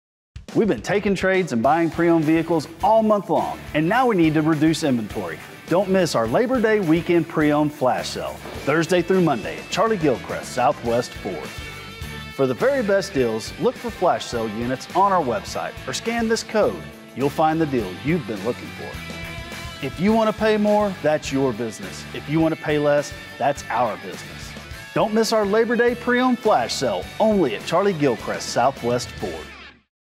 Don't Miss the Labor Day Pre-Owned Flash Sale at SouthWest Ford!
We’ve been taking trades and buying pre-owned vehicles all month long and now we need to reduce inventory! Check out our very best deals on pre-owned cars, trucks, and SUVs! Hurry - sale ends Labor Day 9/5!